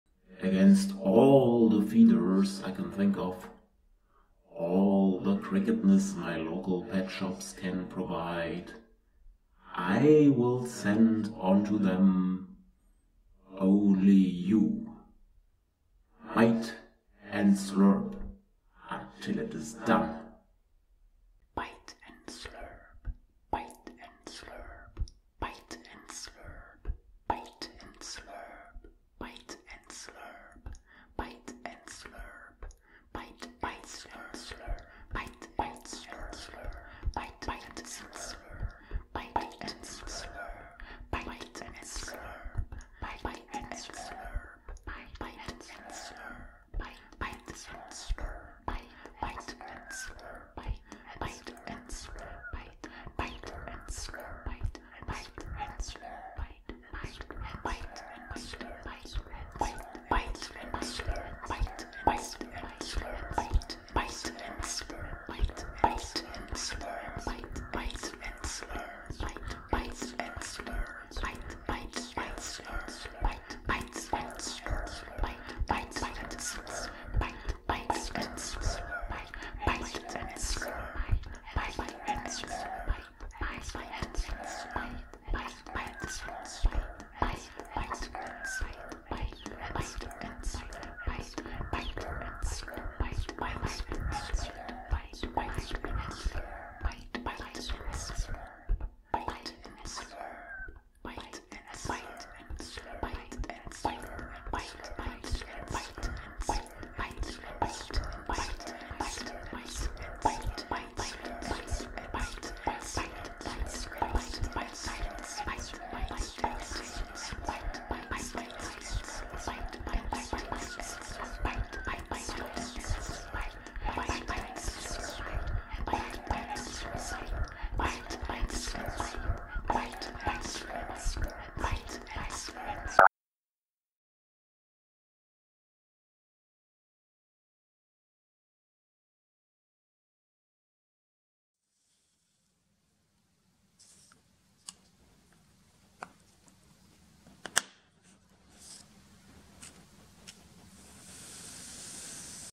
Tarantula feeding | Cyriocosmus elegans | Samia ricini | Bite and Slurp! | Ep. 001
“Against all the feeders I can think of, all the cricketness my local pet shops can provide. I will send unto them, only you. Bite and Slurp until it is done...”

Professor Peppers, my young Trinidad Dwarf Tiger tarantula (Cyriocosmus elegans), teaches you a lesson about being a spider.
Today's topic:
How to handle a silkworm (Samia ricini)